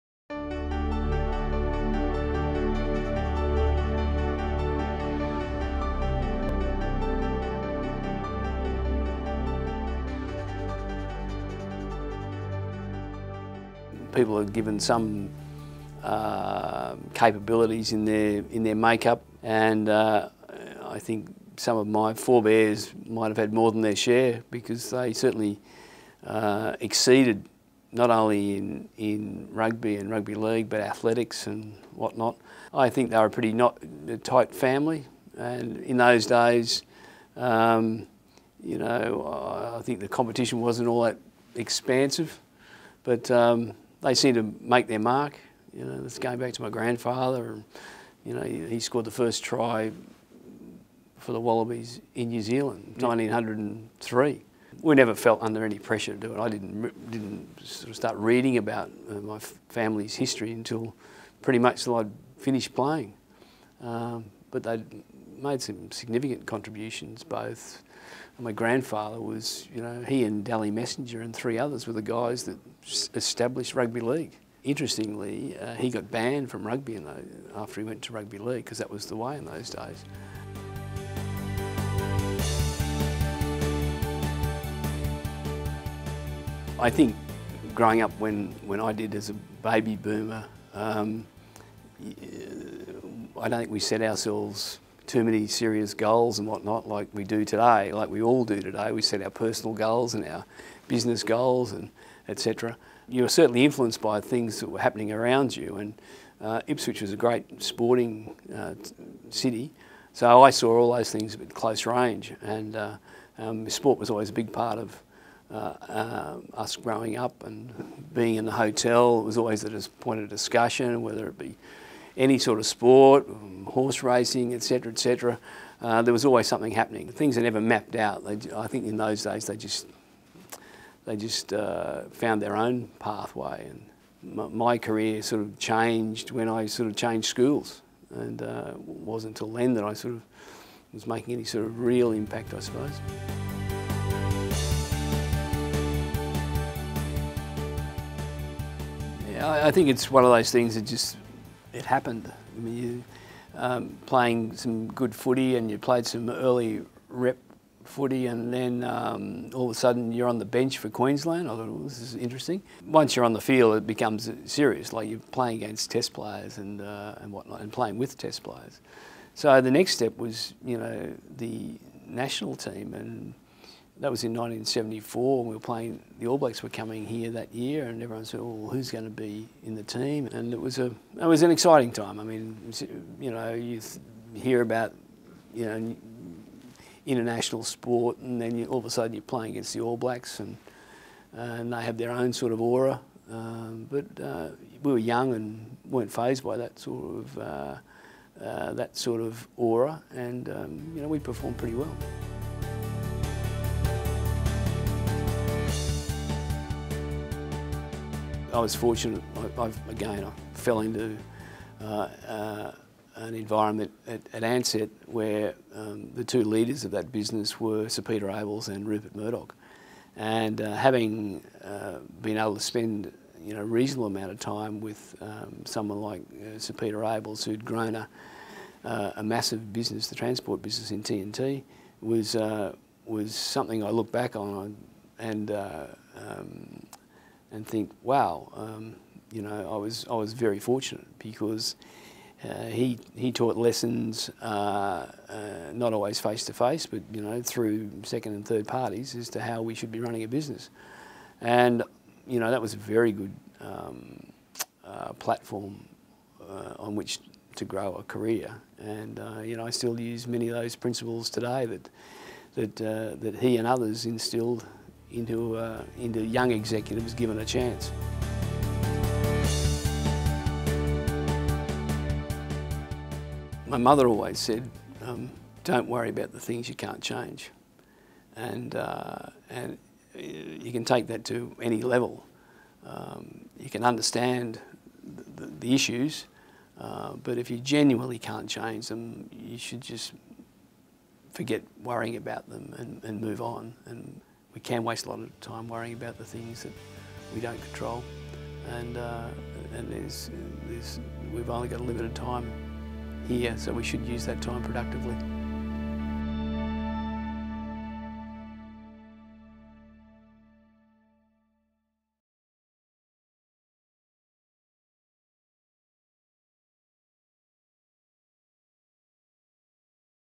Rowland 5 Minute Focus with Paul McLean
Hear from Paul McLean, former Australian former rugby union player and currently CEO Savills Australia and New Zealand.

Visit Rowland.com.au communication, digital and creative agency for more news and information